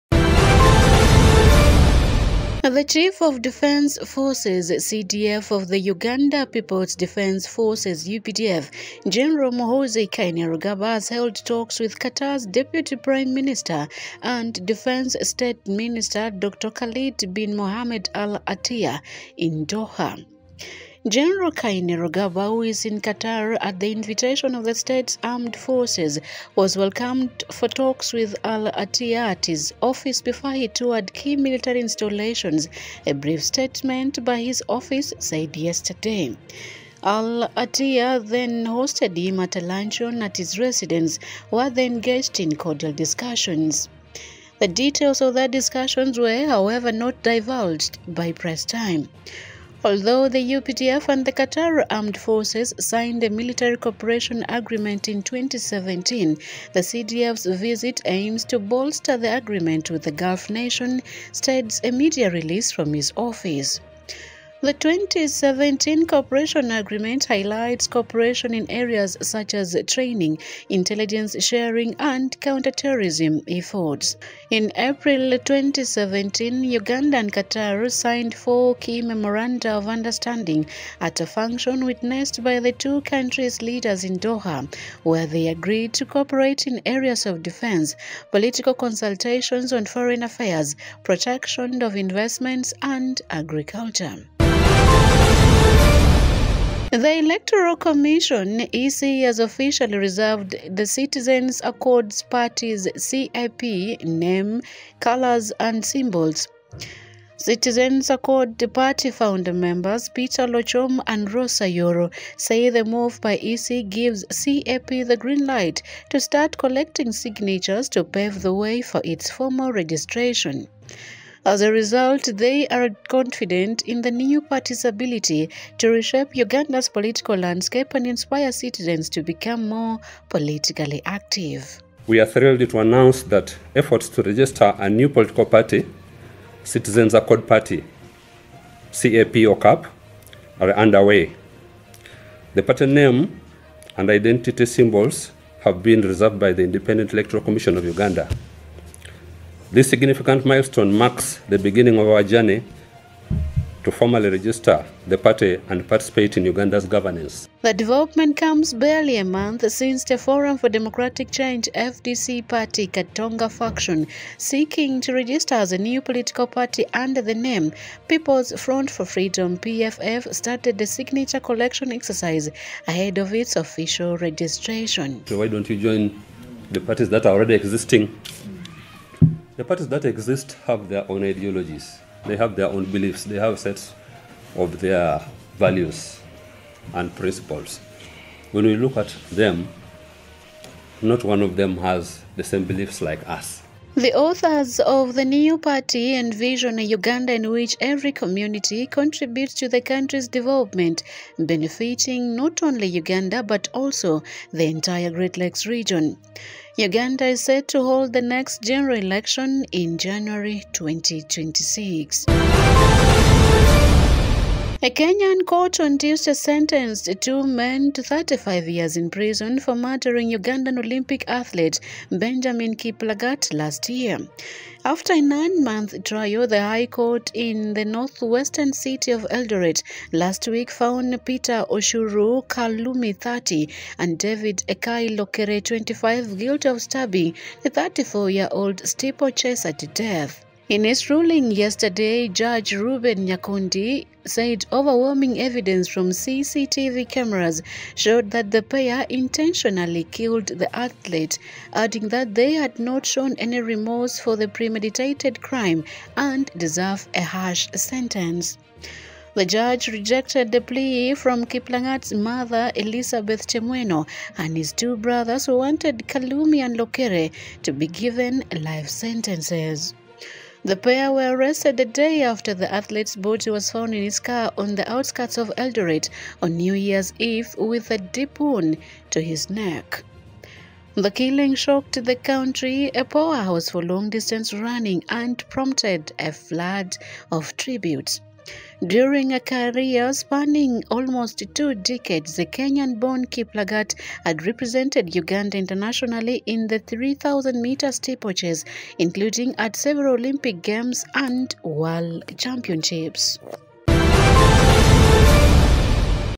Morning DigitalDigest, 06.11.2024
‣ CDF Kainerugaba holds talks with Qatar's deputy premier
‣ New political party: citizens accord
‣ Kenya court jails Kiplagat's killers for 35 years